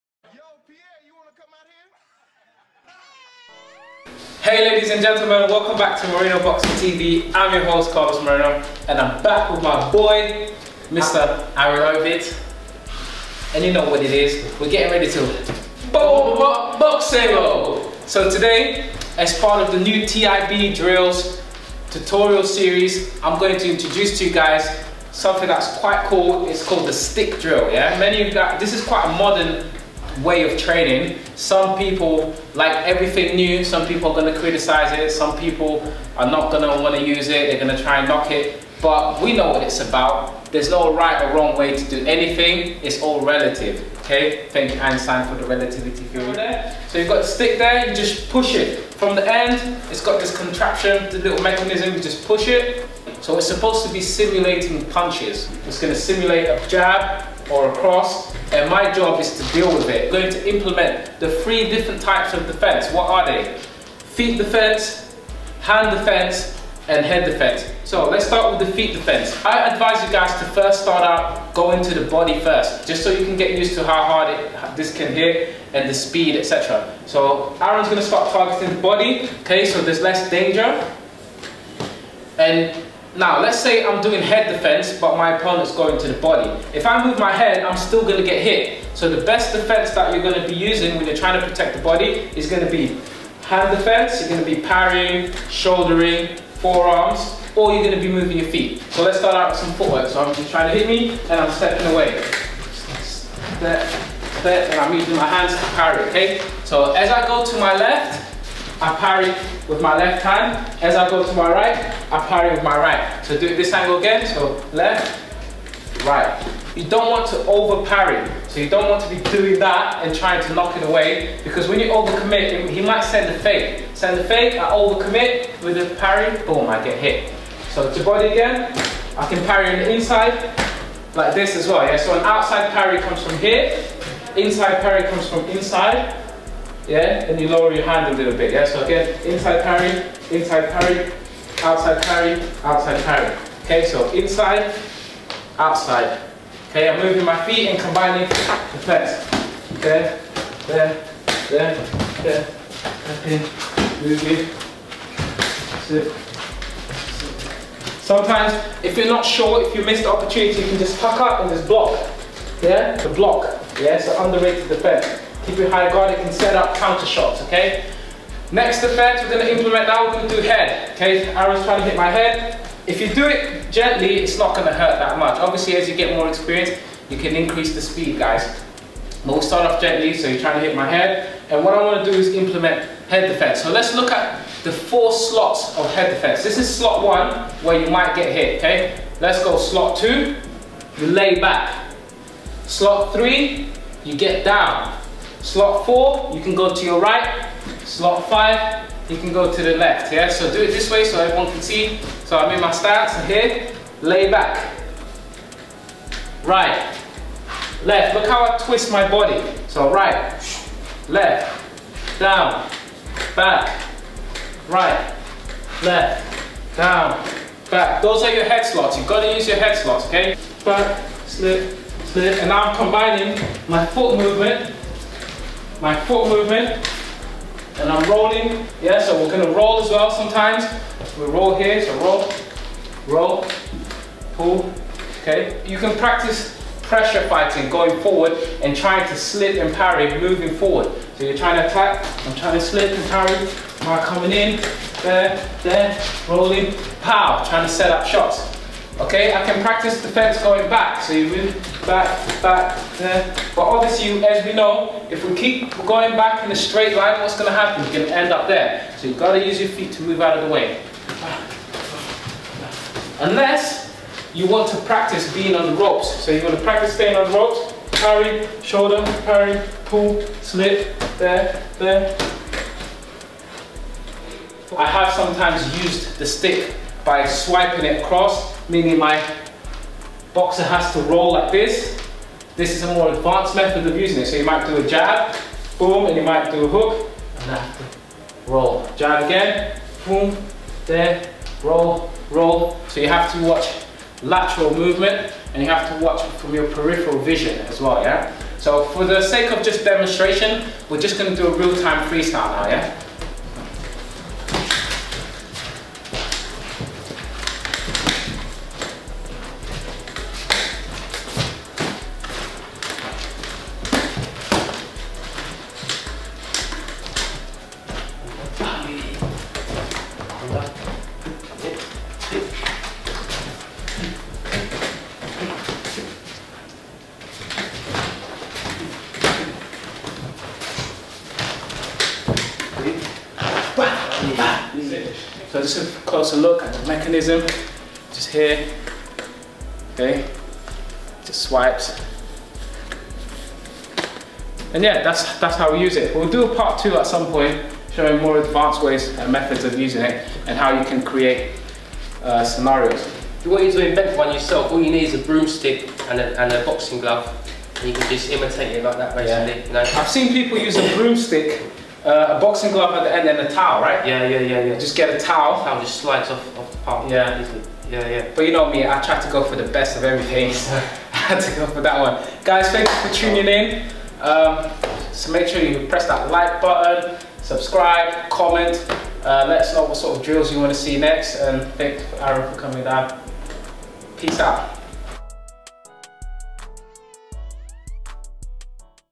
How To Avoid Punches (Head Movement Edition) Moreno Boxing
Another boxing tutorial so soon? Yes indeed! I am aiming to make up for lost time when I did not upload much at all so now you can can enjoy this...

In this tutorial I demonstrate a NEW drill using the 'Boxing Glove Stick!' - you can purchase one online at Sugar Ray's:

I am not sure if they are still selling them but call them and ask!